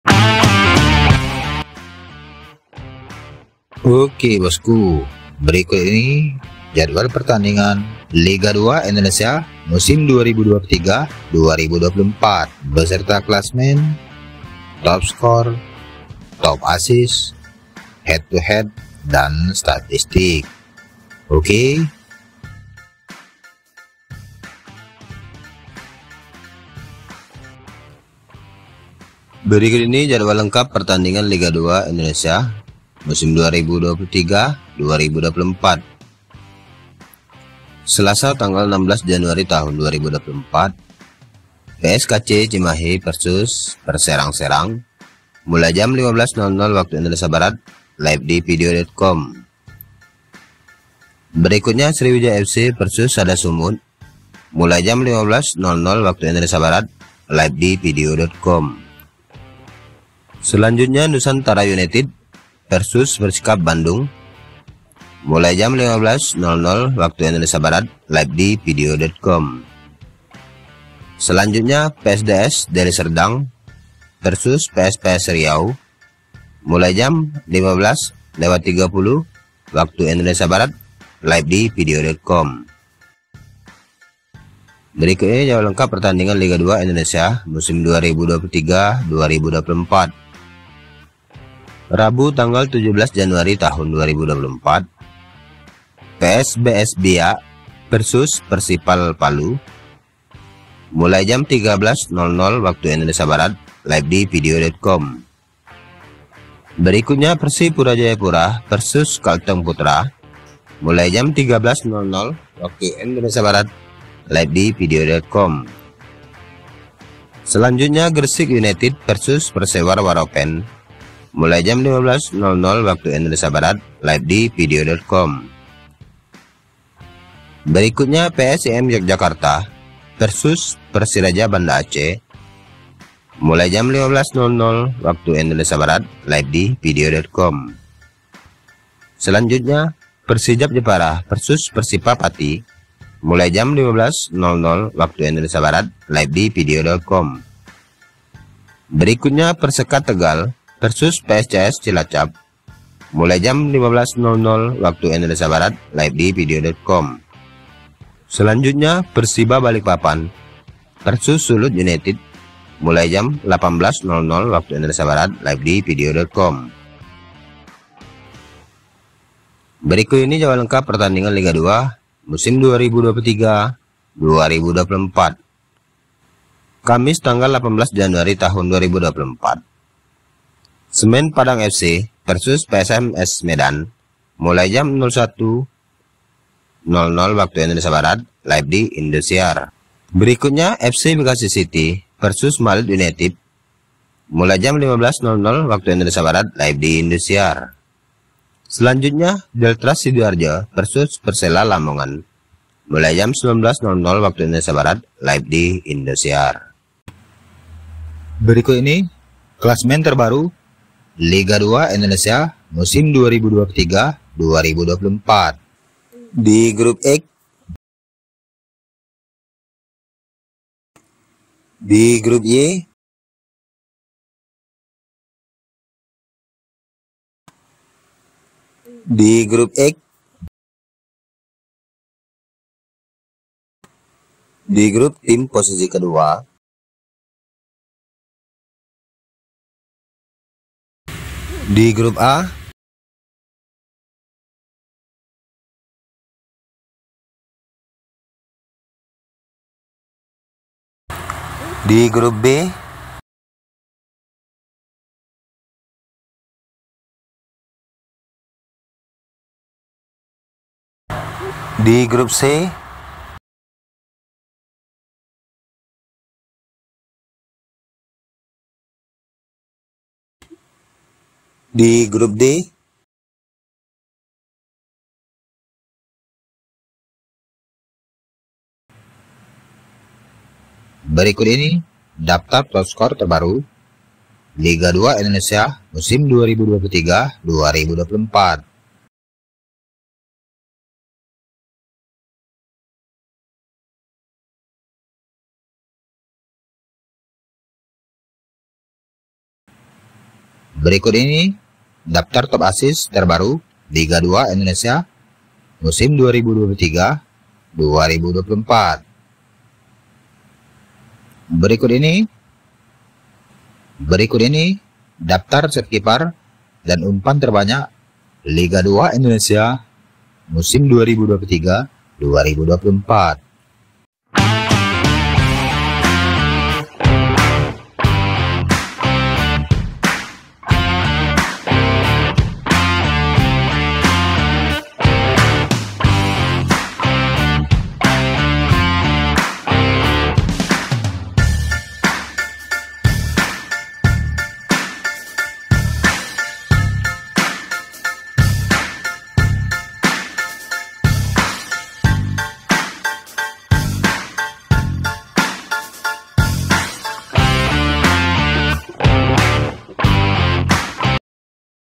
Jadwal 12 Besar Pegadaian Liga 2 2024 - Deltras vs Persela - Bekasi City vs Malut - Live Indosiar
Jadwal 12 Besar Pegadaian Liga 2 2024 -Deltras Sidoarjo vs Persela Lamongan - Bekasi City vs Malut United-  Liga 2 Indonesia 2023 - Live Indosiar
Heat To Head dan Statistik

berikut jadwal 12 besar & playoff degradasi liga 2 2023/2024:
persipura vs persekat
fc bekasi city vs deltras fc
persijap vs persiba
kalteng putra vs pscs
persipa vs sulut united
sada sumut vs perserang
sriwijaya fc vs pskc
malut united vs persela